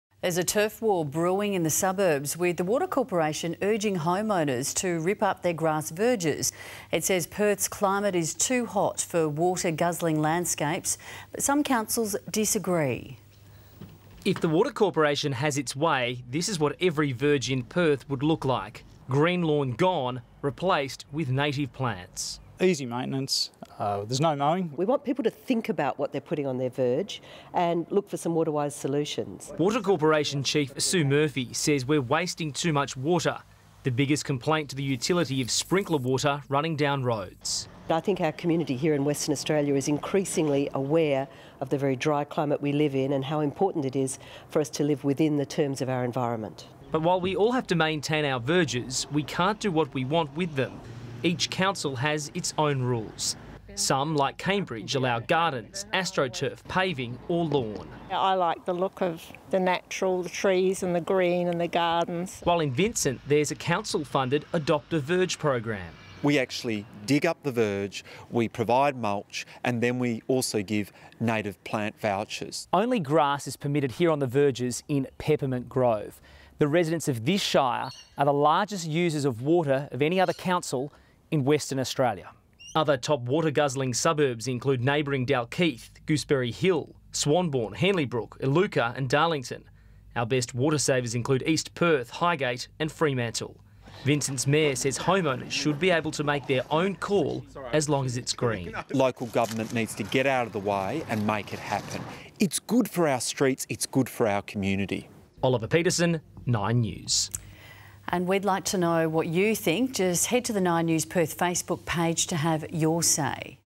Turf Wars | 9 News Perth
There's a turf war brewing in the suburbs with the water corporation urging people to rip up their grass verges.